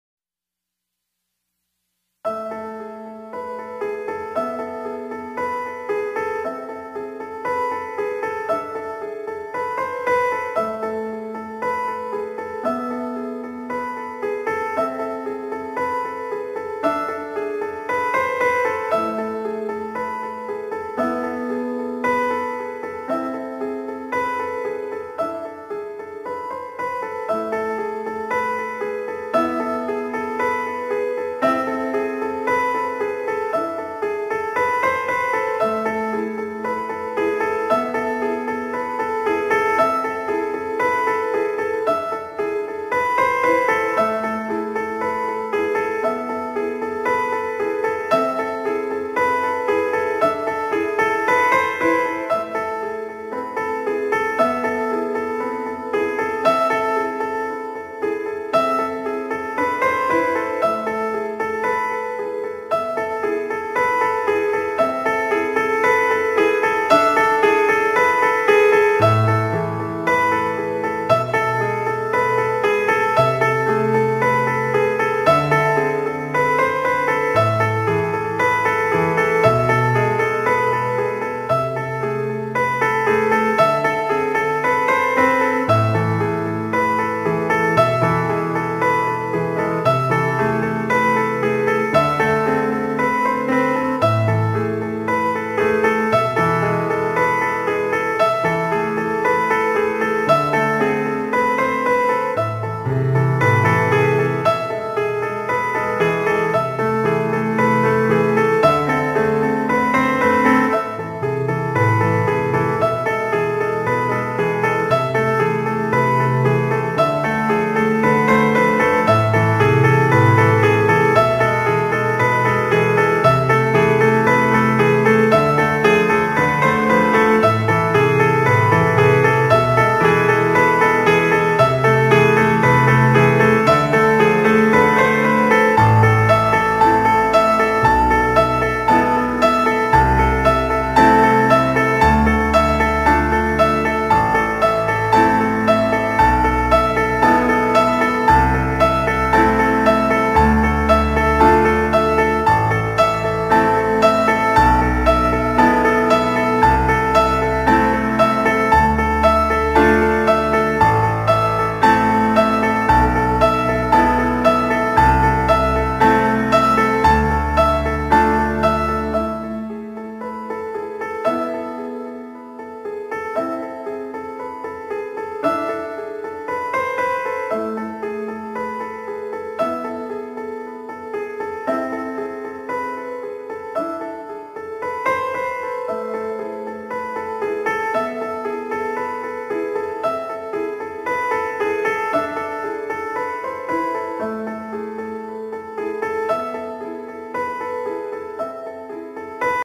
Happy New year Thomas and his friends reflect.
Message: 2016 has been a tough year for me, and probably a lot of people, too. I hope that you all have a happy New year!!